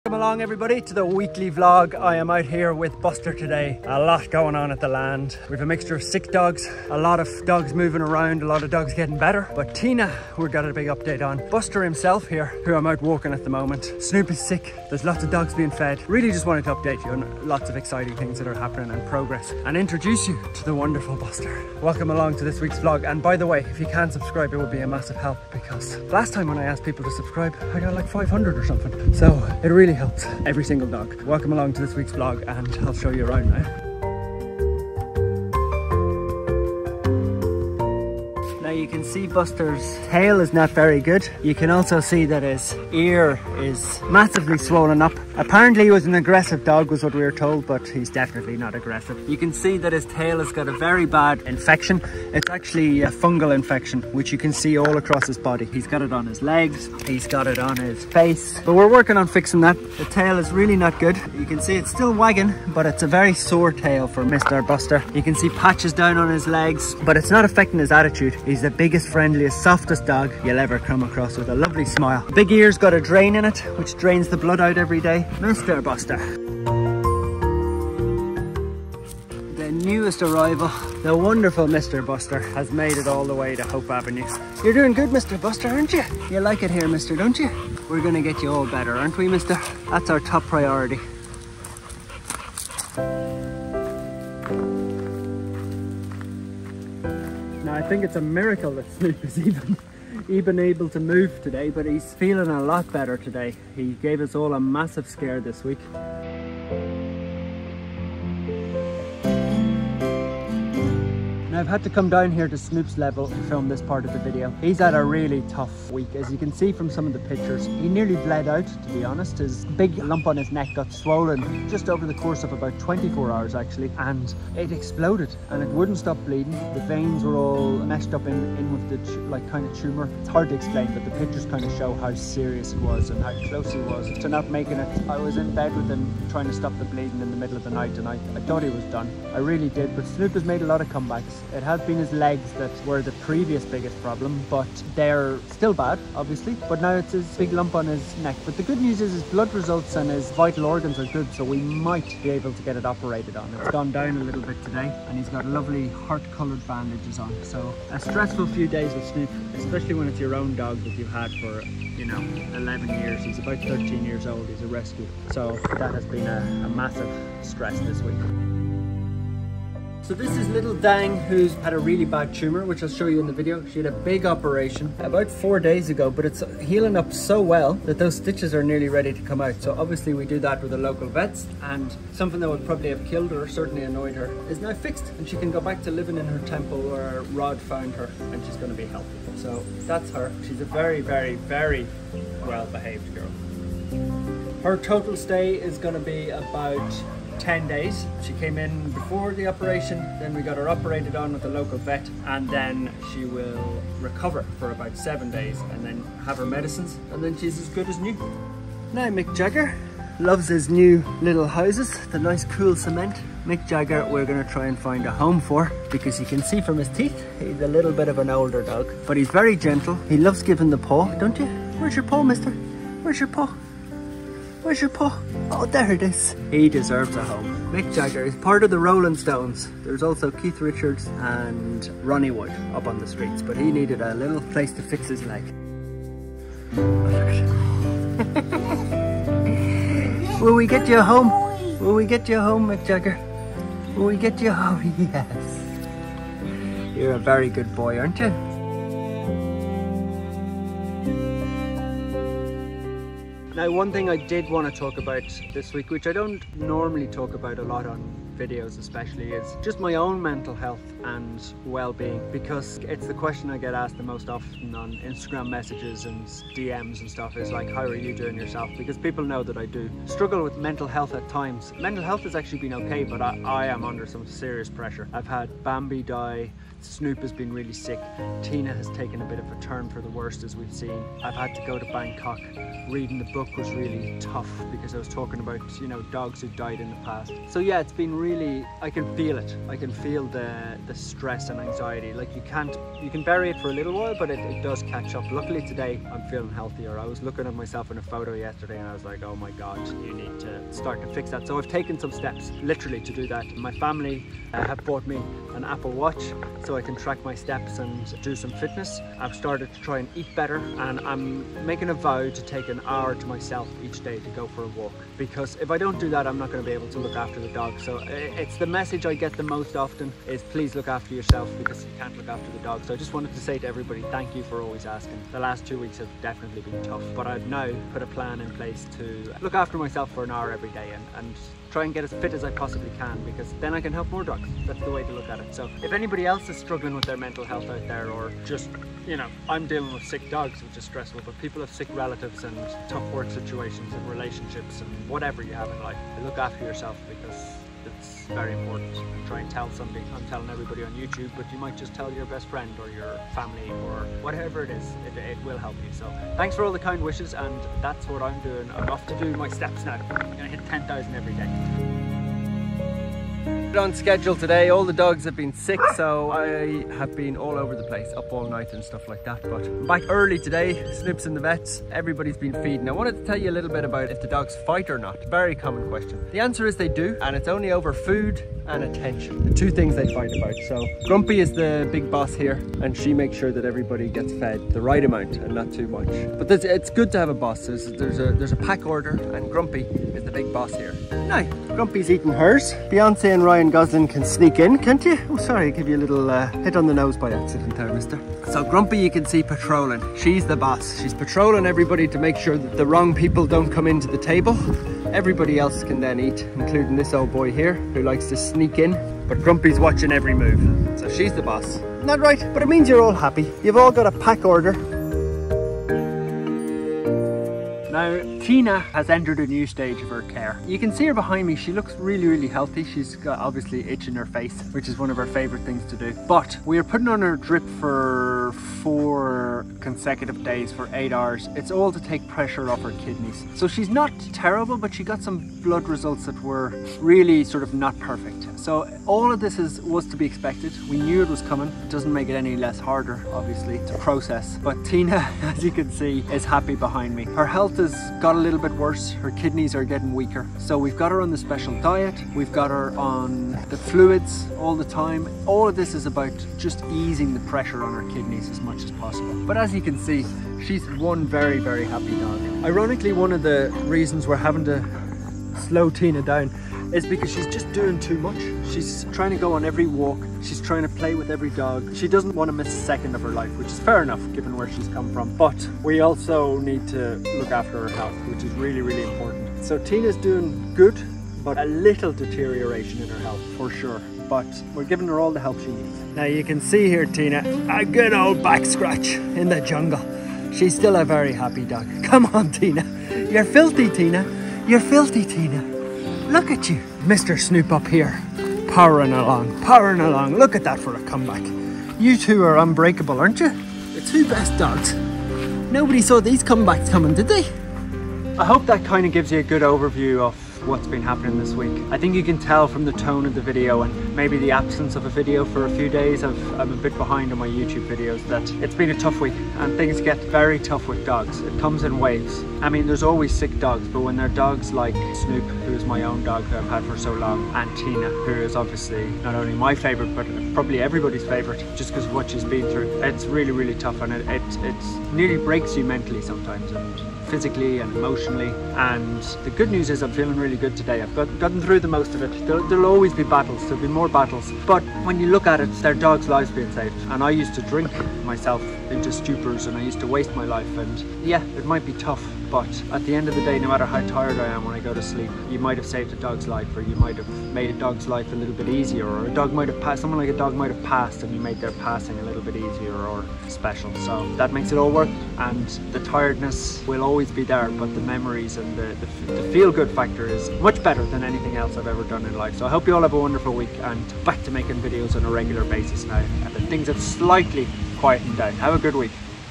New faces needing help, some health scares and the impact of a tough 2 weeks
Niall's weekly vlog catches you up on what's being going on at the land this past few weeks.

Be kind to dogs.